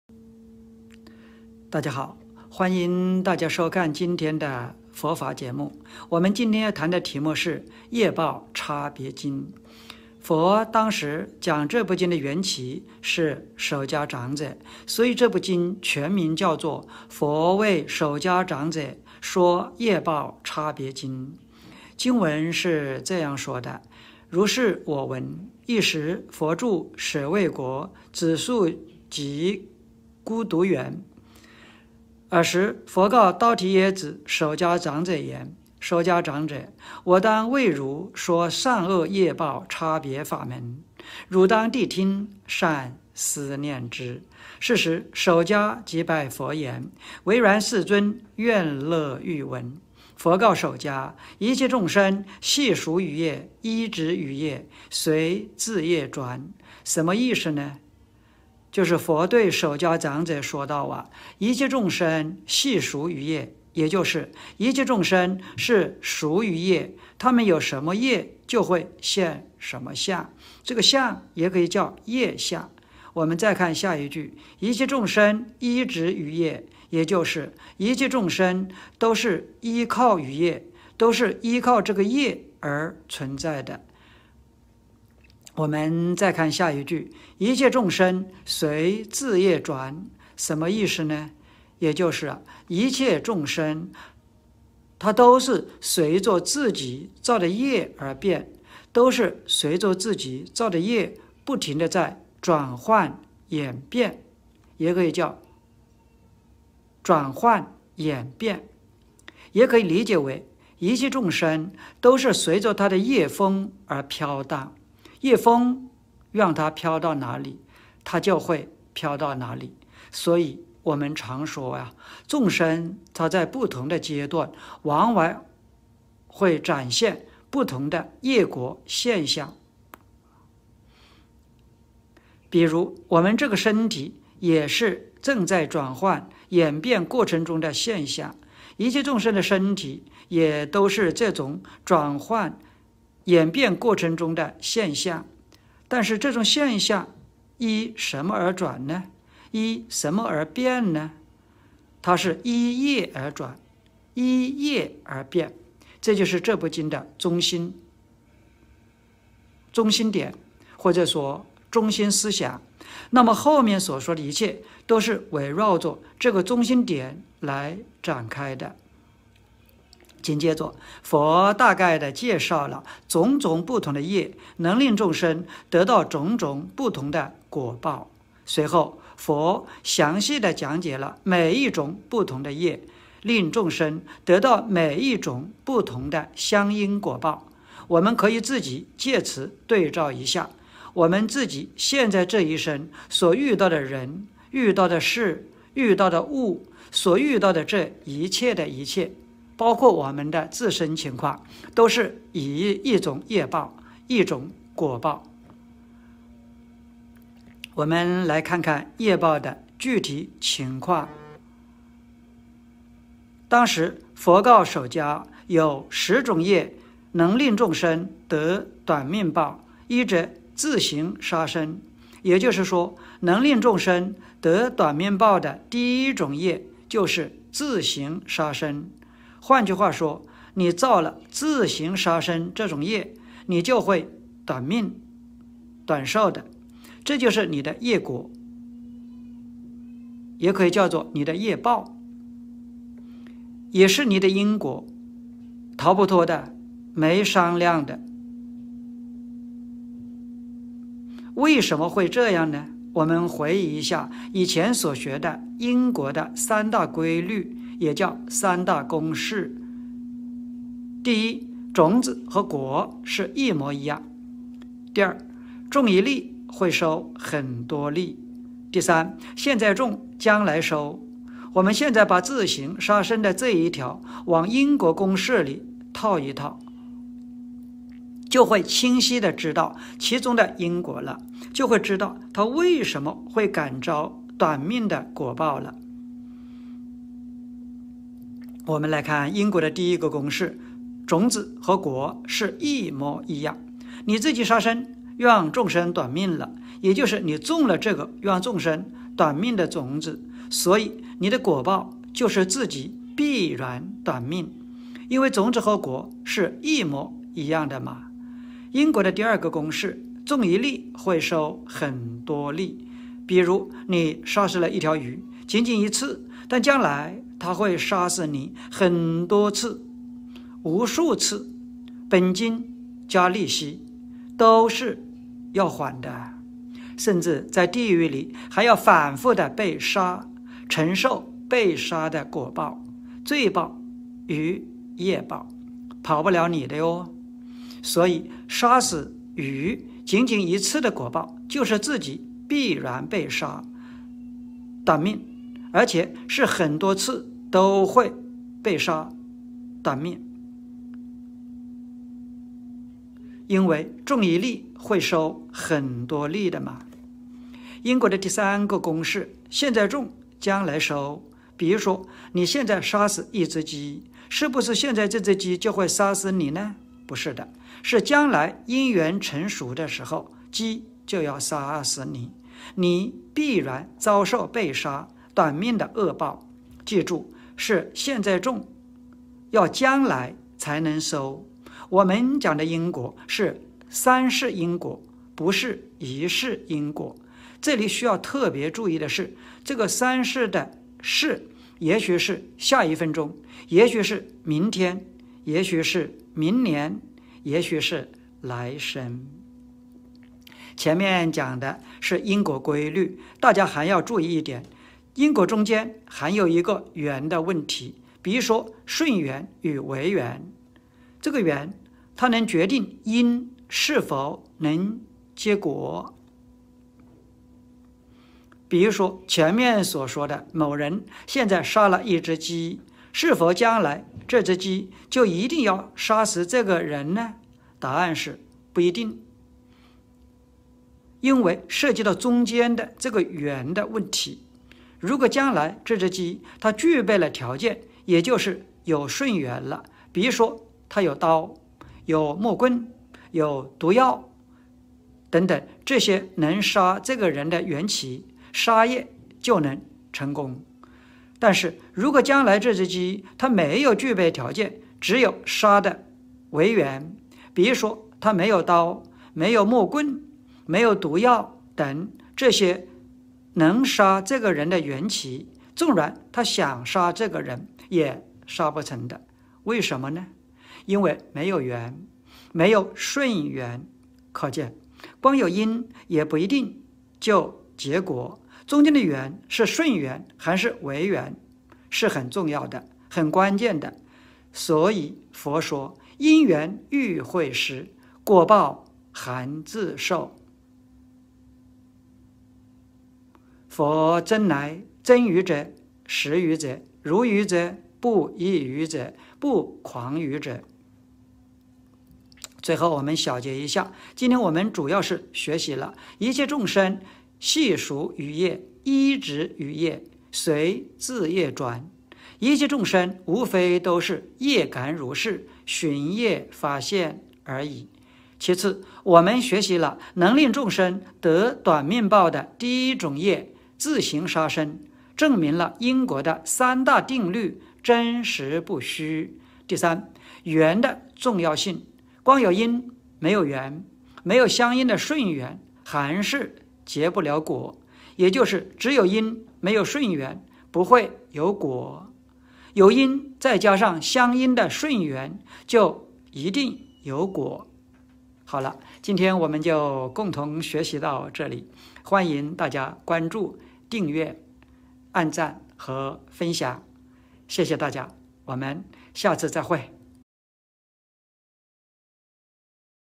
业报差别经的讲解（1）||佛法滋养
佛告首迦：一切眾生，系屬於業，依止於業，隨自業轉。
比如，我們這個身體也是正在轉換演變過程中的現象，一切眾生的身体也都是這種轉換演變過程中的現象，但這種現象依什麼而轉呢，依什麼而變呢？依業而轉！依業而變！
因果的三大规律，三大公式，1.种子和果是一模一样；2.种一粒，会收很多粒；3.现在种，将来收。我们现在把自行杀生这一条往因果公式里套一套就会清晰地知道其中的因果了，就会知道它为什么会感召短命的果报了。